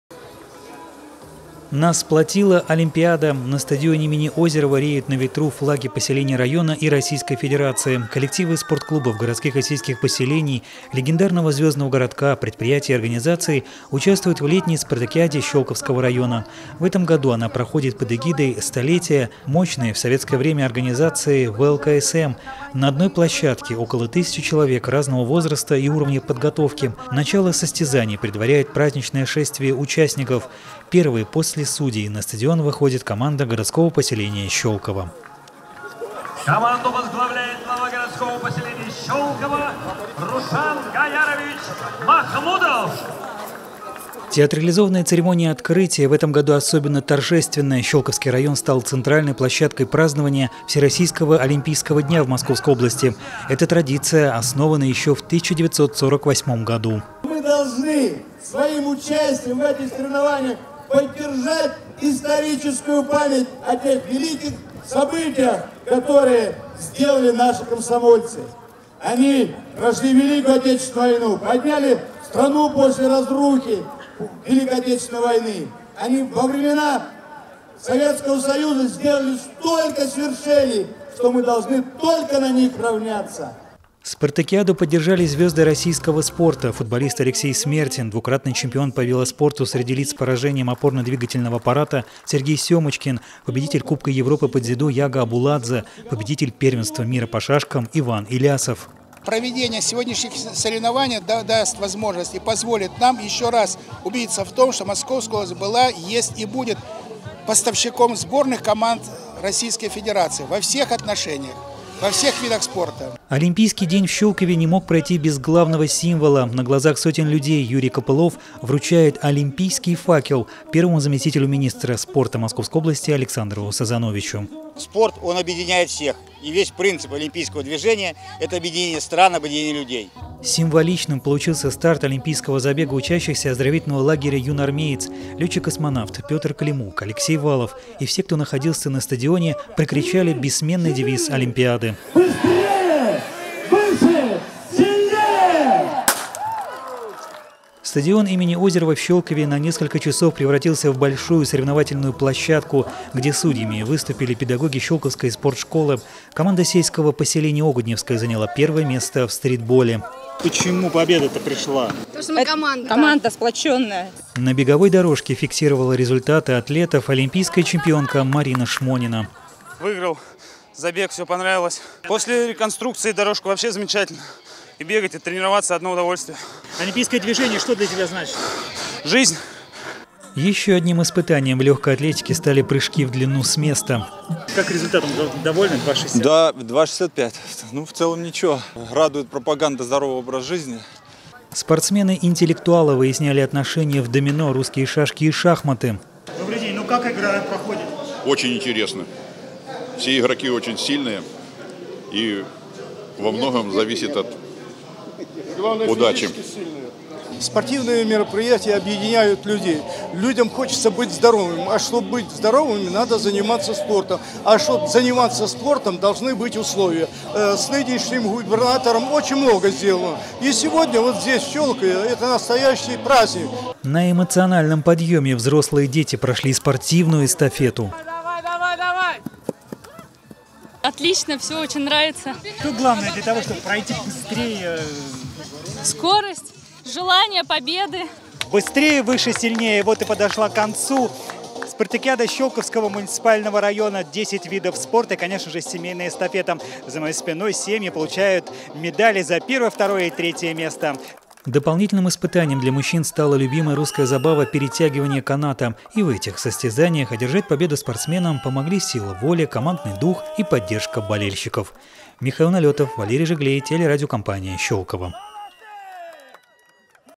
С олимпийским задором
Около тысячи человек приняли участие в Летней Спартакиаде Щёлковского района. В этом году соревнования прошли под эгидой Всероссийского Олимпийского дня и 100-летия ВЛКСМ. О самом массовом спортивном празднике в летнем сезоне – наш репортаж.